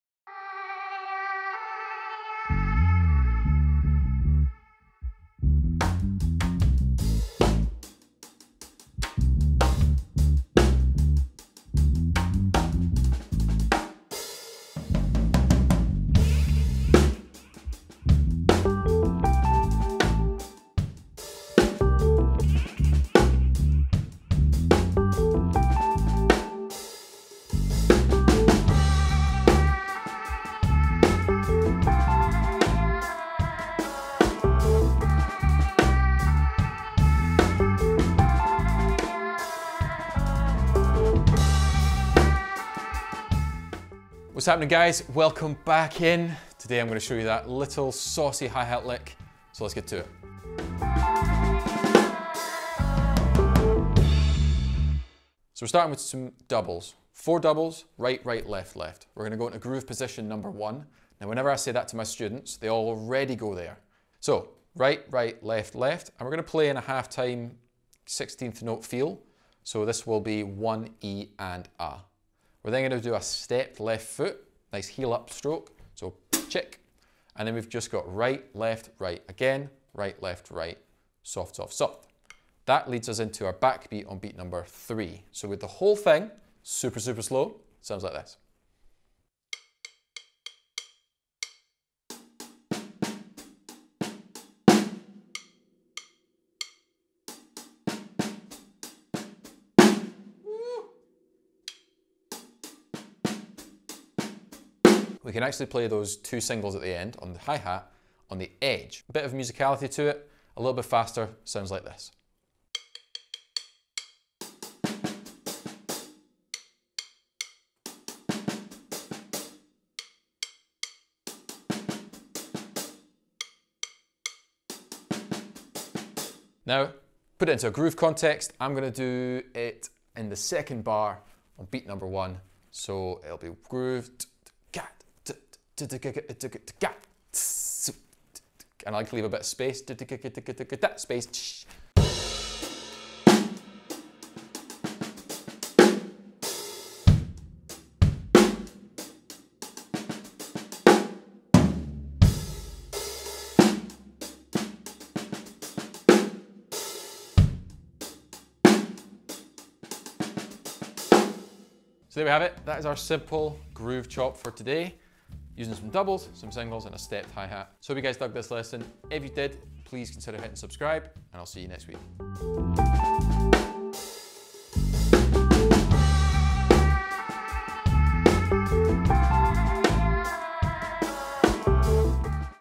Hi Hat Groove Lick Lesson - Free Drum Lesson by Dave Major
I love to Hi Hat licks that just sit in the groove. For this lesson, I'm taking inspiration from drummers like Jost Nickel, Steve Gadd and Ash Soan.

These guys use the hi-hat in such creative ways and especially the stepped hi-hat. The stepped hi-hat is such a cool sound to use in your playing and this groove chop is perfect for just that!

The lick itself is 32nd notes so is going to work really well in a slow half time track such as the one I'm playing in the intro.

Have fun and enjoy this lick!!

D

Drums
Hi hats
Snare top
Snare bottom

Camera
Key light
Led panels

Mics
Snare mic
Kick mic
Lapel mic

If you liked it then share it around. Happy drumming!